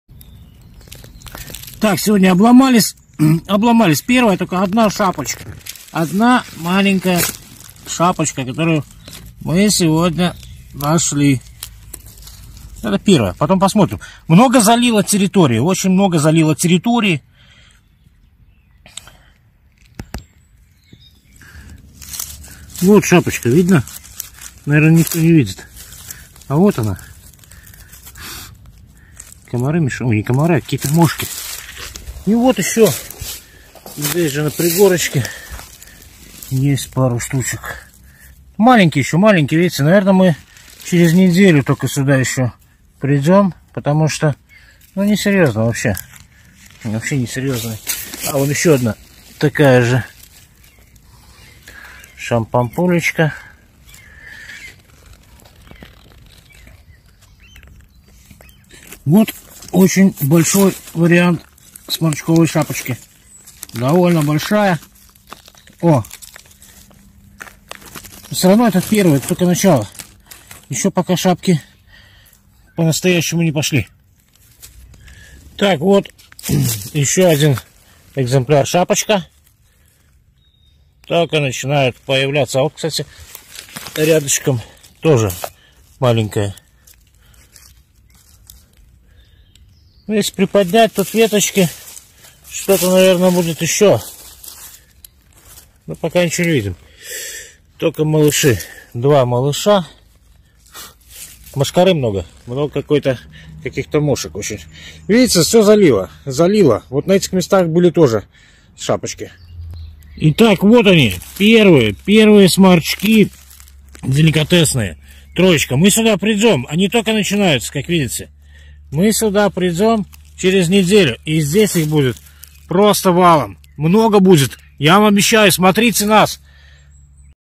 Первые грибы 2023 года. Сморчковая шапочка, сморчки.
Ура, есть первые грибы. Сморчковая шапочка, сморчки показались наружу. Разведка удалась. Через неделю смотрите, будет бум! 💥